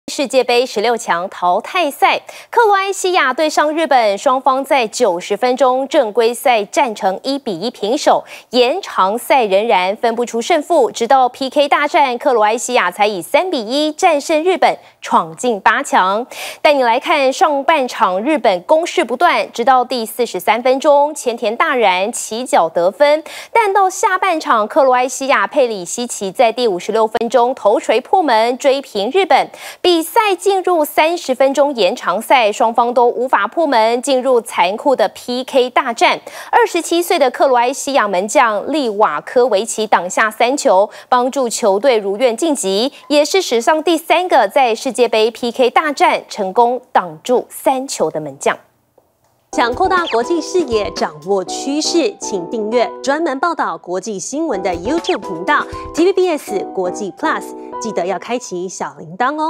PK大戰擋3球 克羅埃西亞3:1勝日本晉八強｜TVBS新聞
克羅埃西亞對上日本，雙方在90分鐘正規賽，戰成1比1平手，延長賽仍然分不出勝負，直到PK大戰，克羅埃西亞才以3比1戰勝日本，闖進八強。